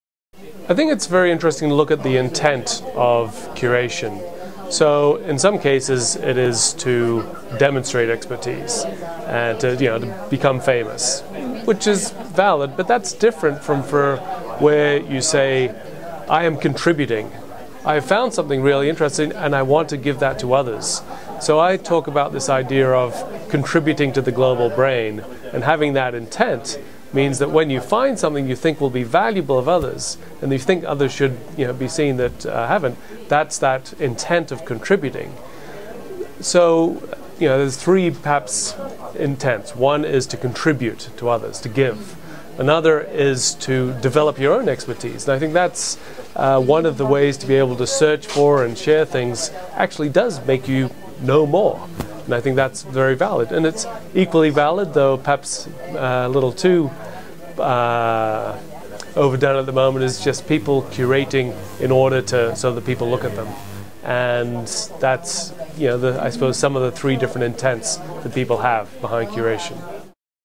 Curation: Is The Intent That Makes All The Difference - Ross Dawson
Intent is the key differentiator when evaluating different types of curation. What is the curator doing it for? Futurist Ross Dawson analyzes why intent is so important in curation.